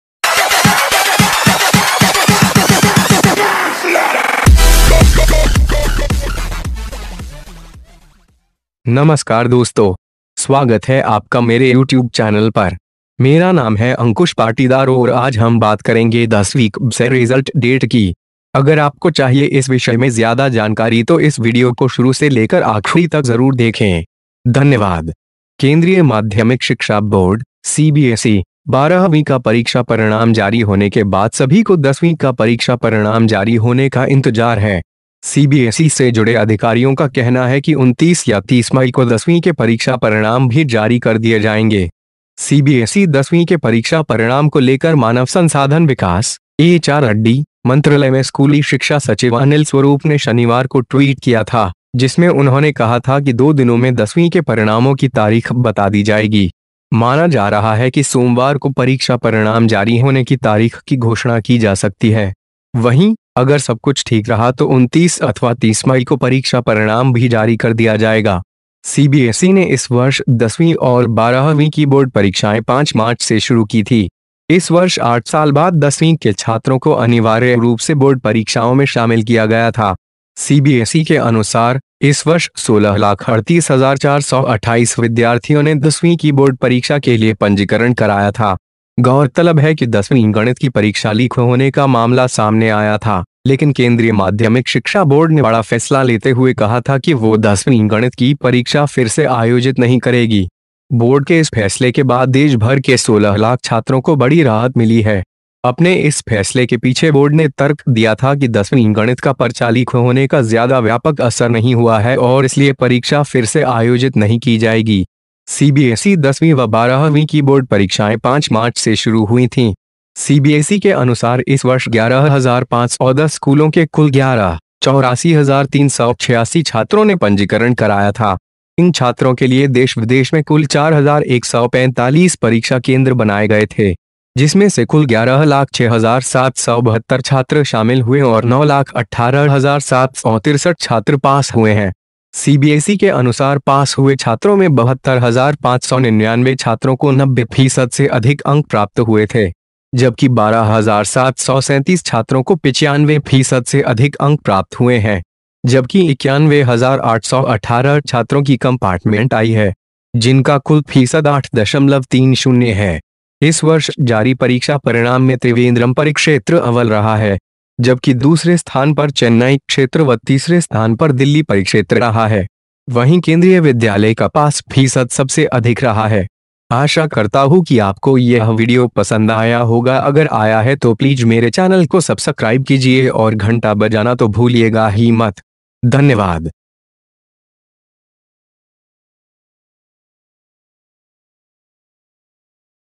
इंतज़ार खत्म आज आएगा 10 वी का रिजल्ट,सब पास होंगे,CBSE Announced Result Date For Class 10,2018
CBSE 10th result 2018 : Like every year, the Central Board of Secondary Education is ready to conduct the 10thstandard Board exams in the Month of March and April. After the exams, the appearing students will be able to check the CBSE 10th Result 2018 on official site. To download the result page you can check the result by adding your roll number and name. Considering from the last year’s reviews, this year it is expected that the board will announce the result CBSE 10th Exam Result 2018 in the 1st week of June. Every student can check their results on the official